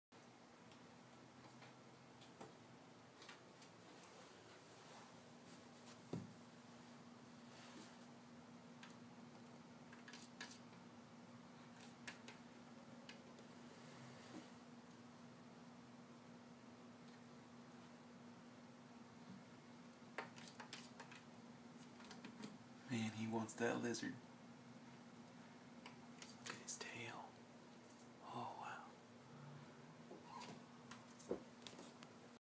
Leo wants that lizard
Leo tries to get a snack.